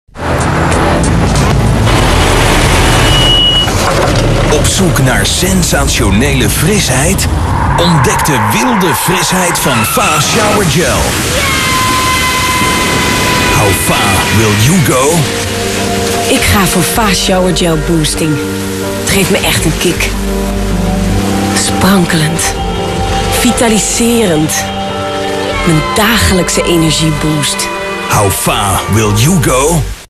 RTV Digital Media - Eindproduct FA - Fa : Brandpaal - Reclame 1
Voor FA shampoo deze Voice Over ingesproken door onze vaste stemacteur Peter de Gee. Ook zo'n fantastische zwoele voice over boeken? Kijk op de website van Peter de Gee